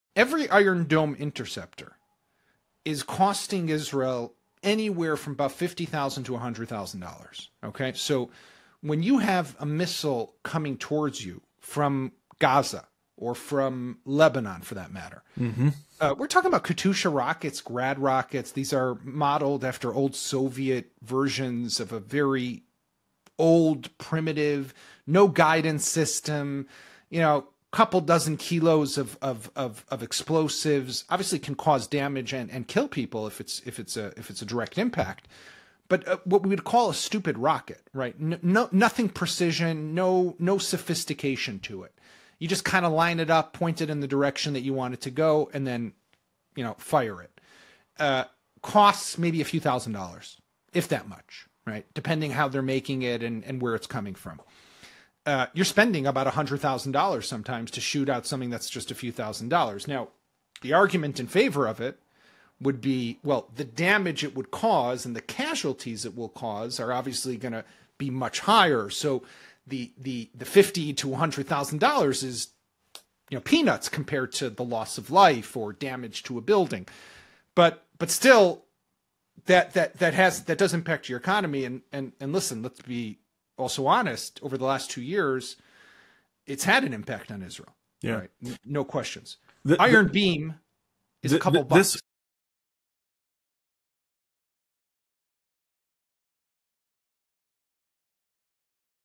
The Insane Economics of Missile Defense (Until Now)
Defense expert Yaakov Katz breaks down the unsustainable economics that have plagued missile defense for decades. He explains how Israel was forced to spend $50,000-$100,000 on interceptors to stop crude rockets costing just a few thousand dollars—and how Iron Beam's $2-per-shot laser technology finally solves this critical problem.